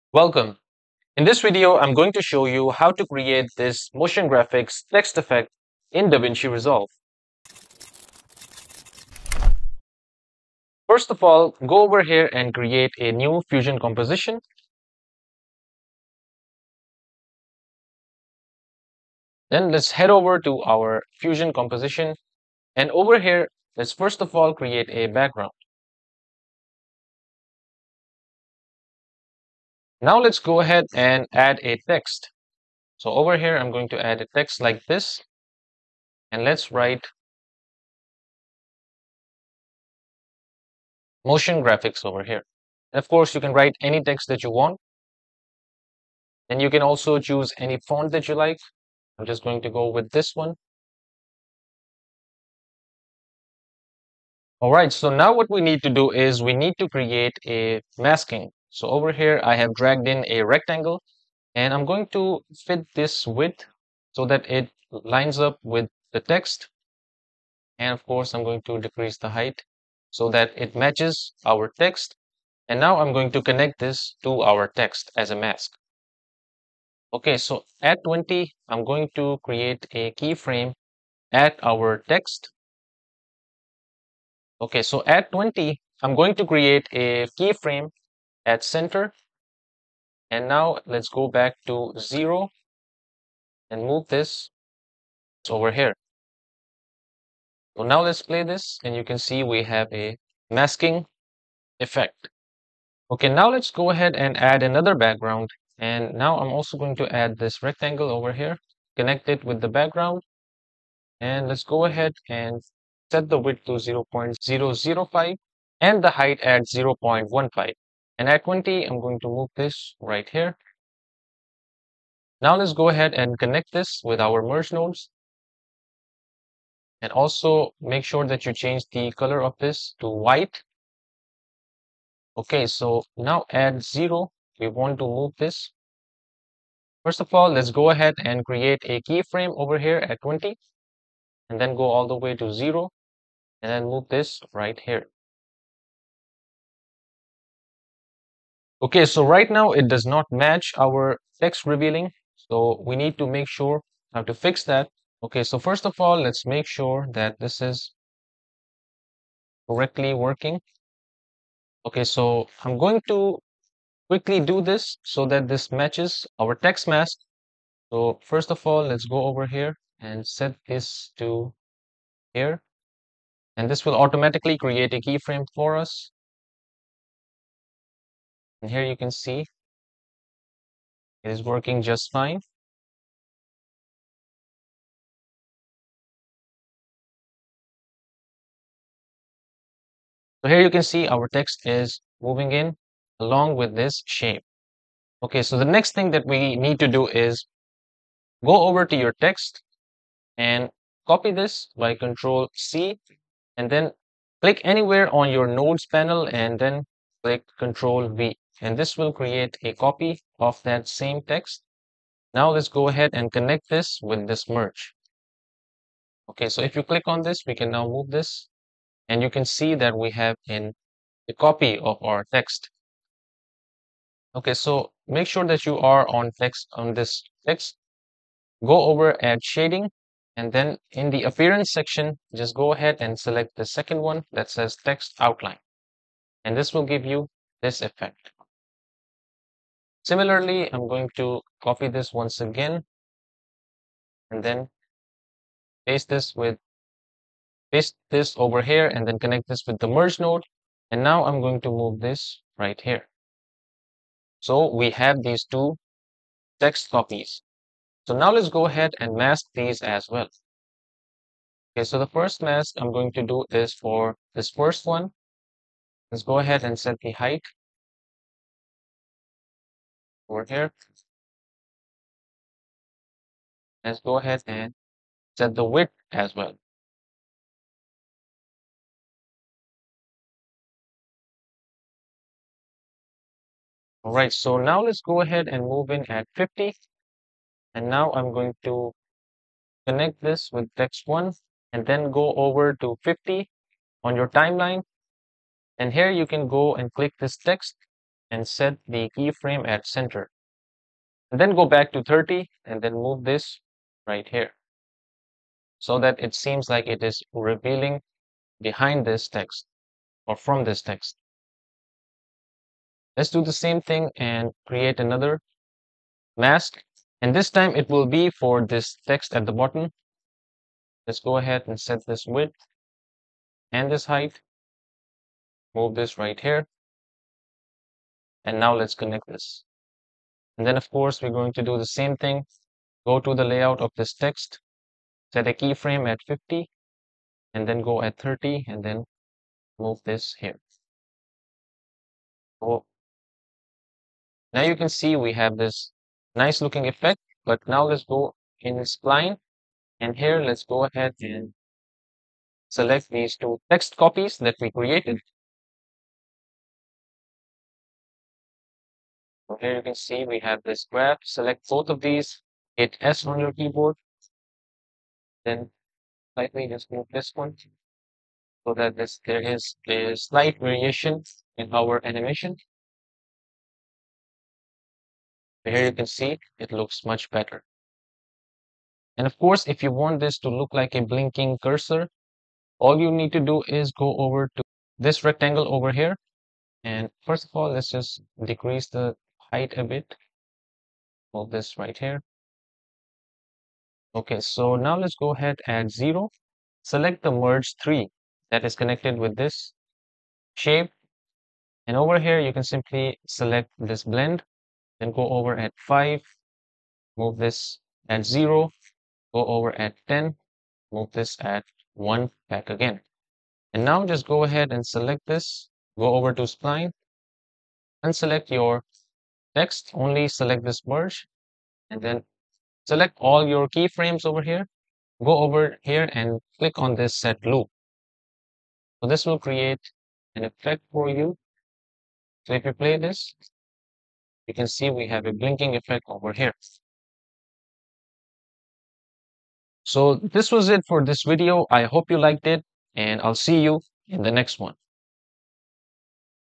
Davinci Resolve 18 Text Effect (Motion Graphics Tutorial)
In this video you will learn how to animate text with effects. Minimalistic modern text motion graphics.